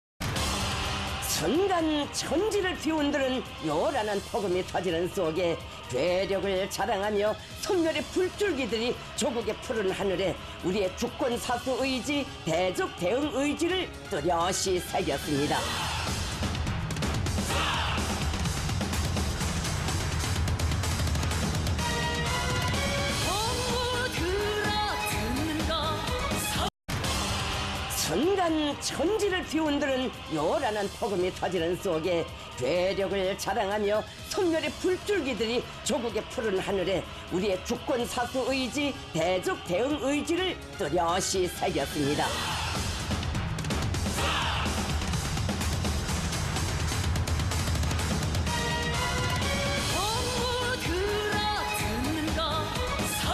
North Korea launched a barrage of 18 KN-25 missiles
North Korea launched a barrage of 18 KN-25 missiles. It appears that the video cuts out right as half of them start veering off course. KN-25 is a SRBM with a demonstrated range of 380 km, officially described as a "super-large calibre" multiple launch rocket system.